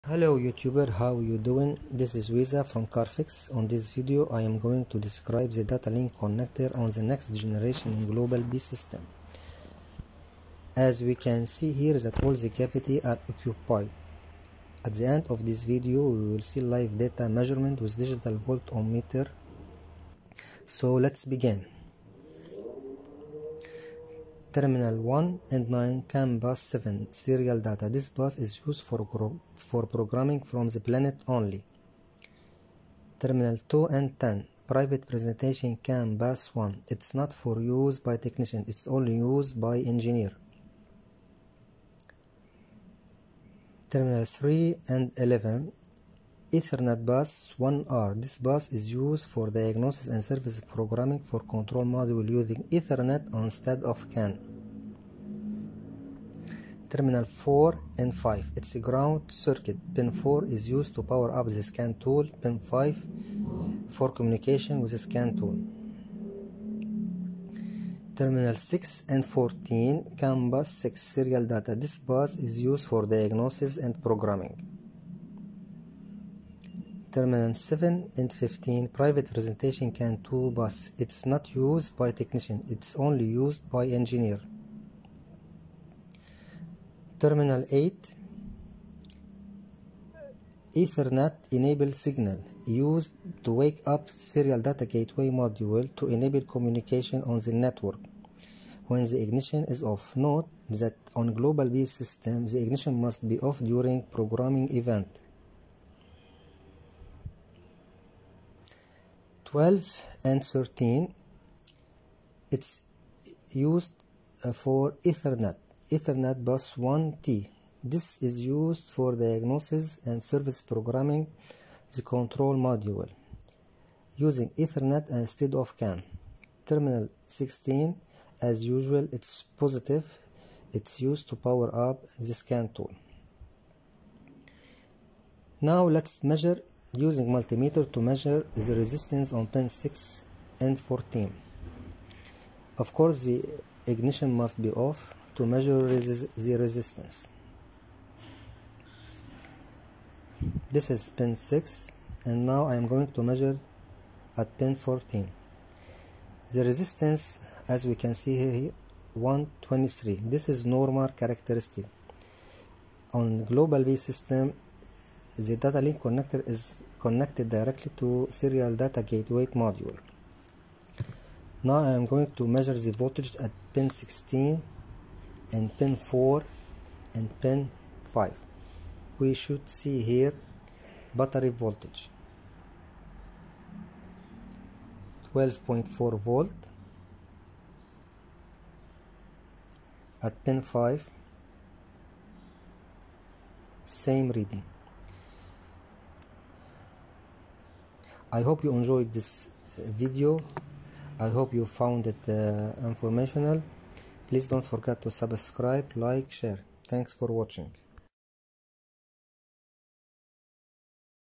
Global B architecture Data Link Connector Description
Terminals 1 & 9 CAN Bus 7 Serial Data this bus is used for programming from the plant only
Terminals 2 & 10 Private Presentation CAN 1 bus is not for use by a technician it's only used by Engineering
Terminals 3 & 11 Ethernet Bus 1R This bus is used for diagnostics and service programming of control modules using Ethernet instead of CAN.
Terminals 4&5 is a ground circuit pin 4 is used to power up a scan tool and pin 5 is for communication with the scan tool
Terminals 6 & 14 CAN Bus 6 Serial Data this bus is used for diagnosis and programming
Terminals 7 & 15 Private Presentation CAN 2 bus is not used by a technician it is only used by Engineering
Terminal  8 is Ethernet Enable Signal and is used as wakeup SDGM to enable the communication on the network when the ignition is OFF (Note that on the global B system the ignition must be OFF during programming.
Terminals 12 & 13 are used for Ethernet bus Ethernet Bus 1T This bus is used for diagnostics and service programming of control modules using Ethernet instead of CAN.
Terminals 16 is B+ Positive and it's used to power up the scan tool as the old version.